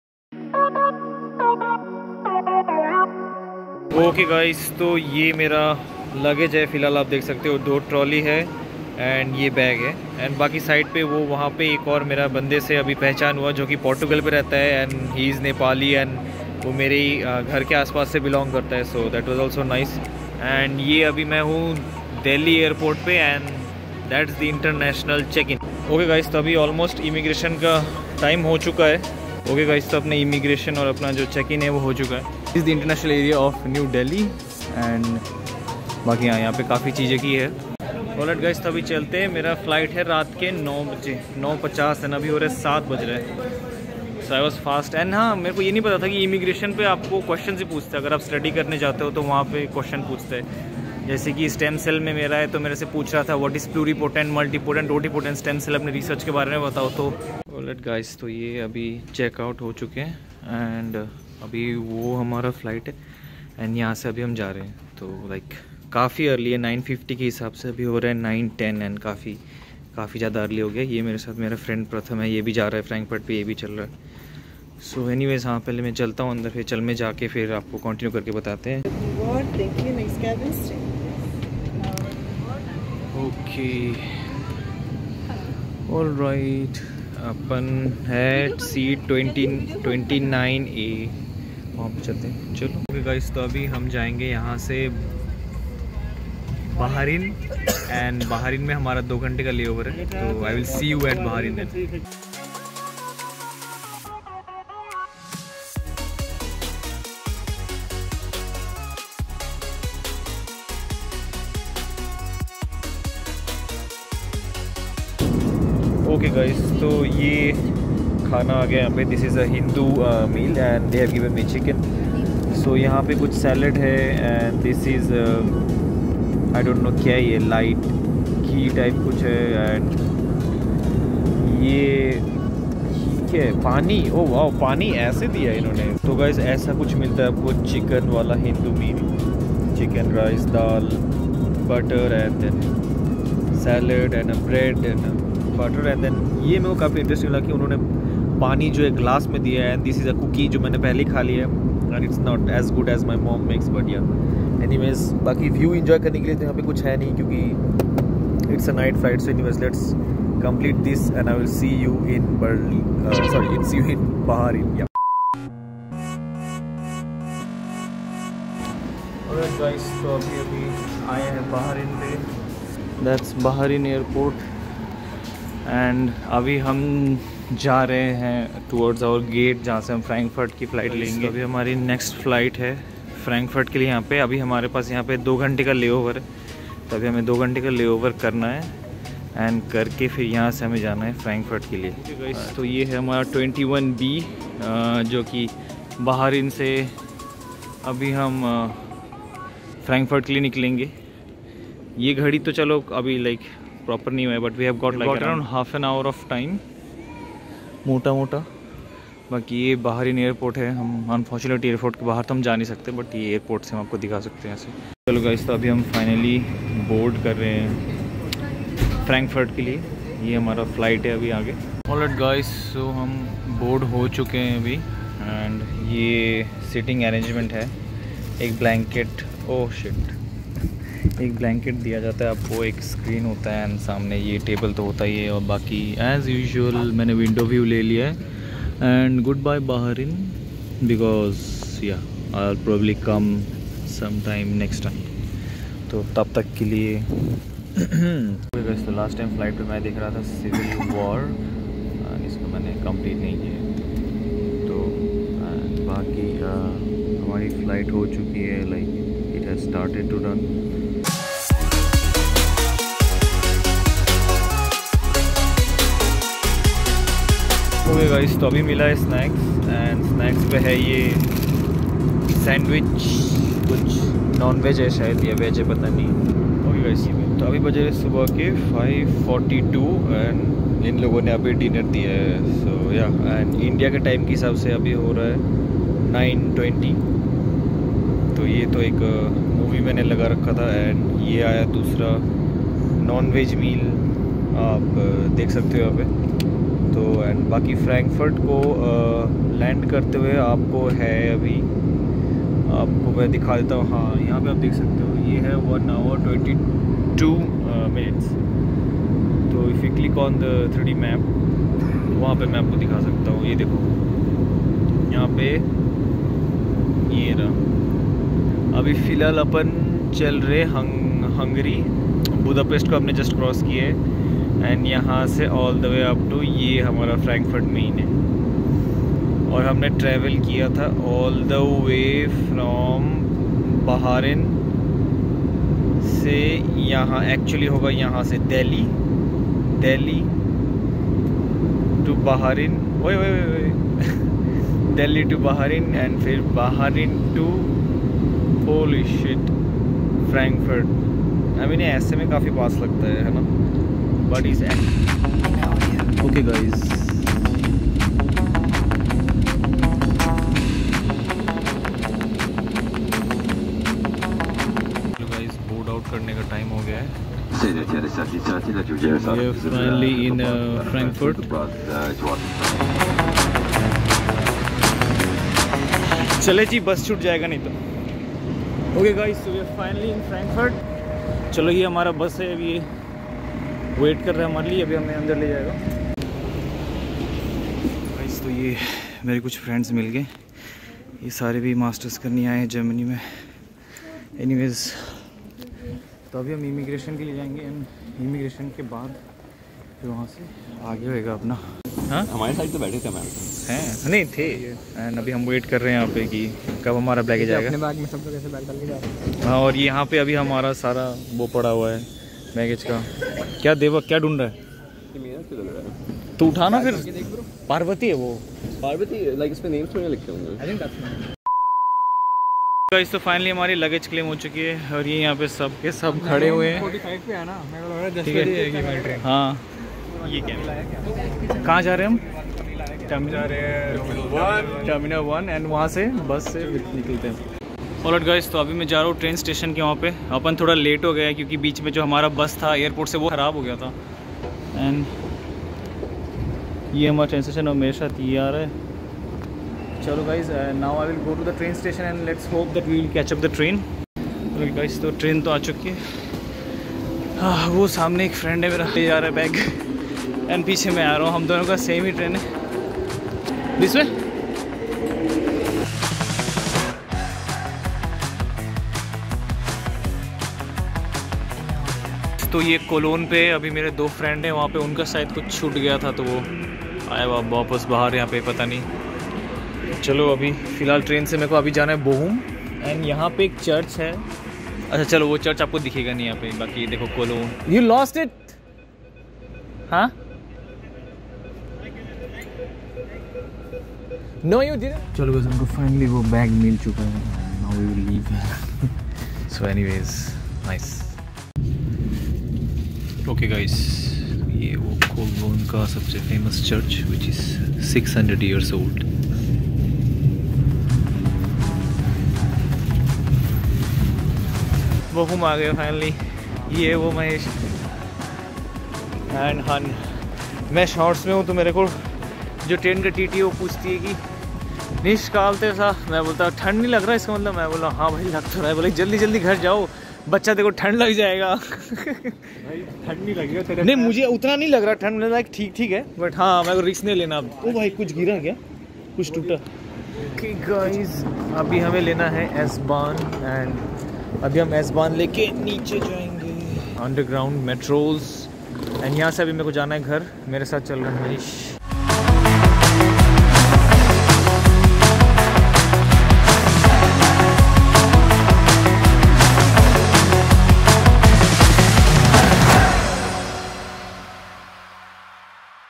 Delhi to Frankfurt - Masters In Germany | Fresh Start In Student Life
So I finally went to Germany , I'am documenting my life and journey through the world and my student life ...

More Vlogs On The Wayyy ...